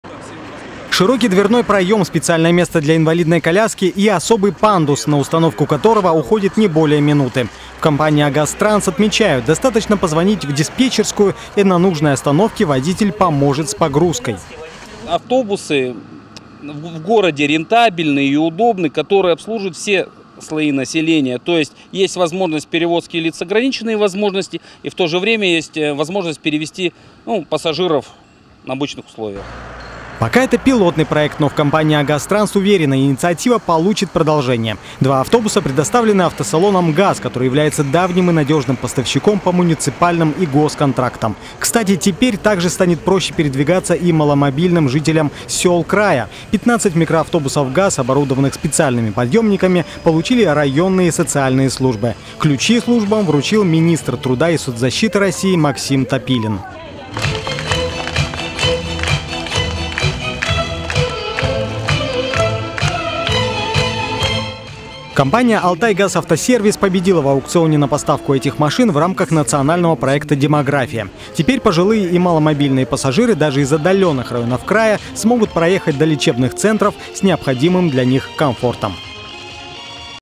Маломобильные жители Барнаула и края теперь смогут ездить на специальных автобусах с пандусами
В августе в Барнауле по 77-му маршруту начнет ездить сразу два автобуса, оборудованных специальными пандусами. Это поможет маломобильным гражданам, которым нужно, к примеру, попасть в краевую больницу, добраться до места с максимальным комфортом и совершенно бесплатно.